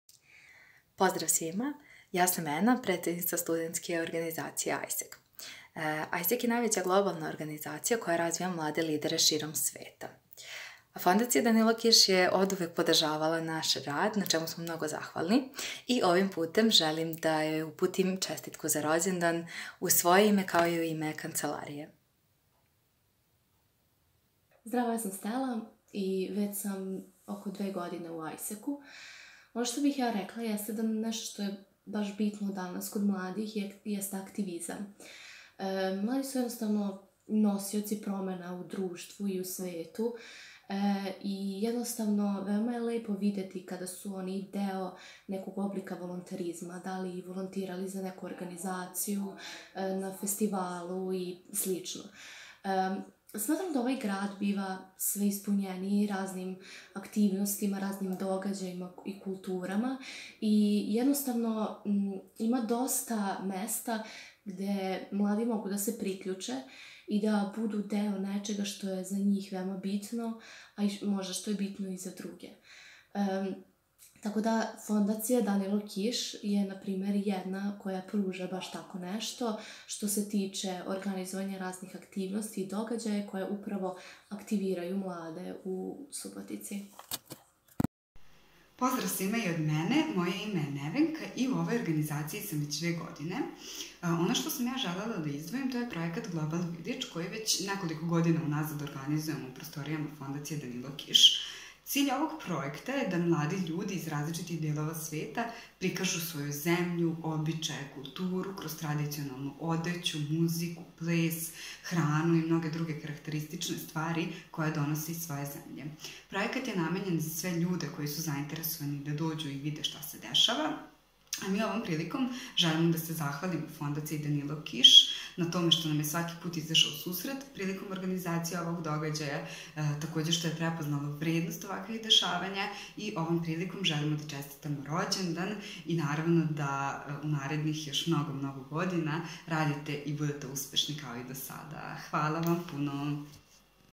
Rođendanska poruka: Ena Stipić, Stela Nemčev, Nevenka Ćirić
Ispred omladinske organizacije AIESEC Ena Stipić, Stela Nemčev i Nevenka Ćirić svojom porukom čestitale su rođendan Fondaciji.